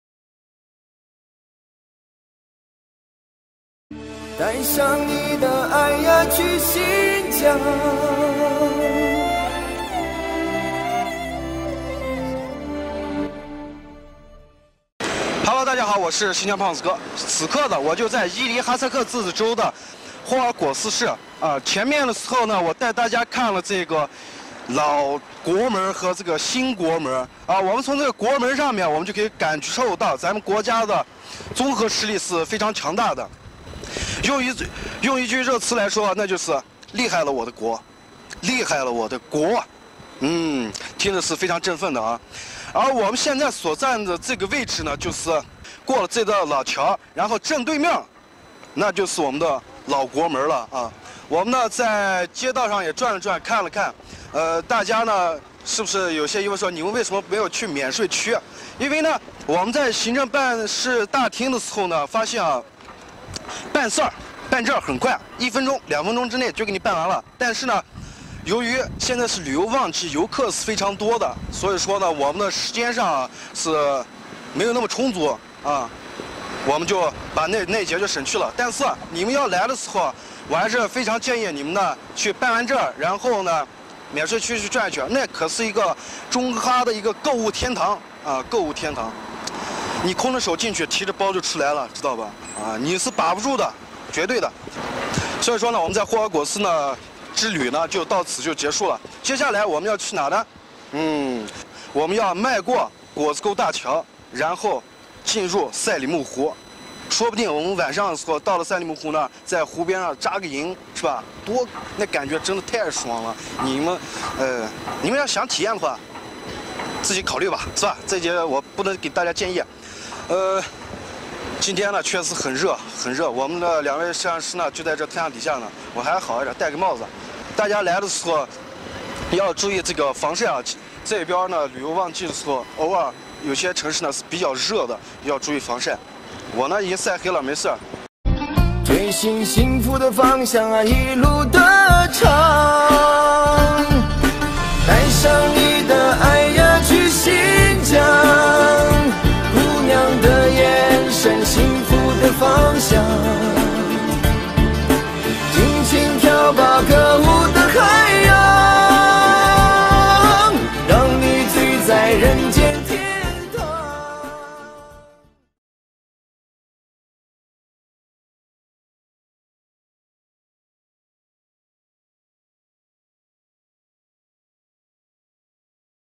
【北疆游】#26 胖纸哥来到新疆口岸之首 霍尔果斯口岸新老国门震撼
霍尔果斯口岸是新疆口岸之首，今日胖纸哥一行来到了新老国门，真彻感受到厉害了我的国